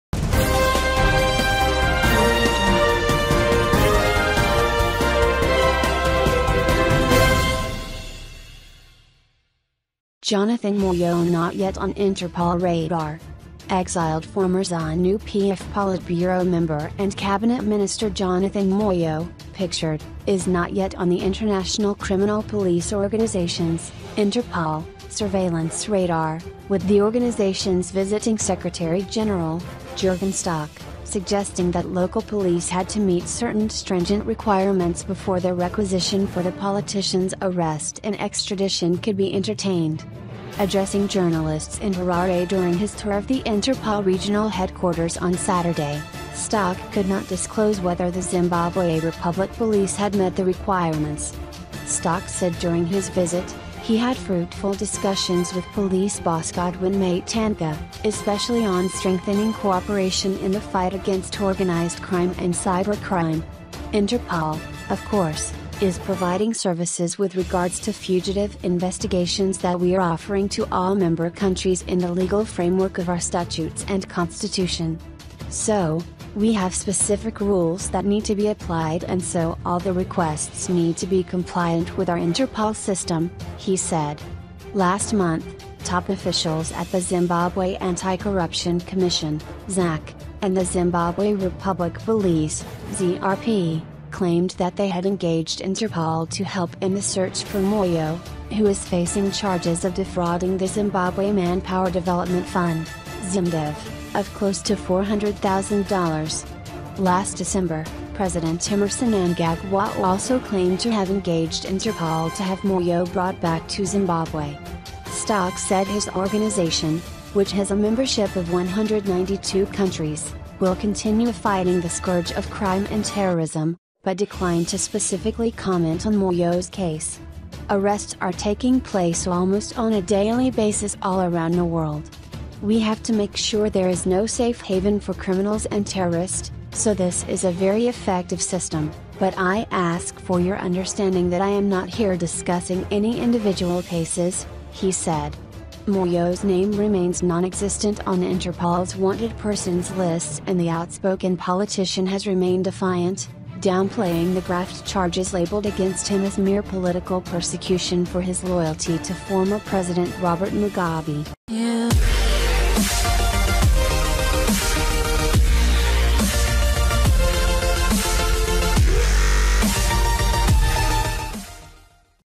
Jonathan Moyo not yet on Interpol radar
EXILED former Zanu PF politburo member and Cabinet minister Jonathan Moyo (pictured) is not yet on the International Criminal Police Organisation’s (Interpol) surveillance radar, with the organisation’s visiting secretary-general, Jürgen Stock, suggesting that local police had to meet certain stringent requirements before their requisition for the politician’s arrest and extradition could be entertained.